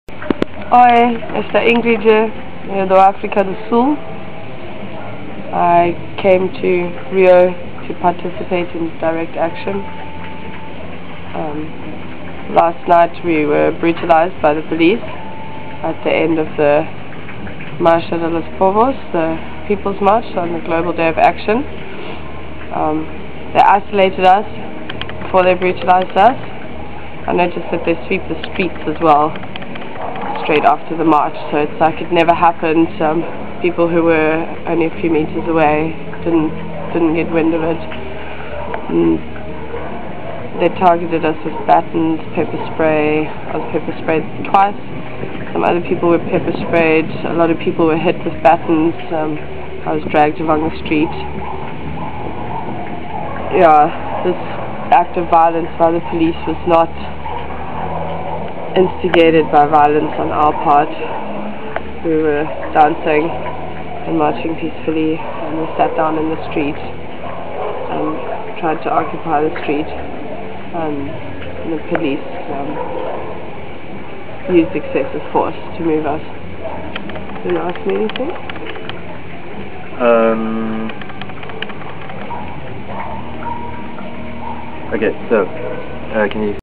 UNneccesary Polica brutality in Rio at the Marcha do Povos
Ingrid D chats about her experiences of UNneccessary polica force at the end of the Global Day of Action march in Rio. As part of the Marcha do Povos (Peoples March), Ingrid was in a group that was kettled and attacked by the local police force, getting punched, hit by batons and pepper-sprayed.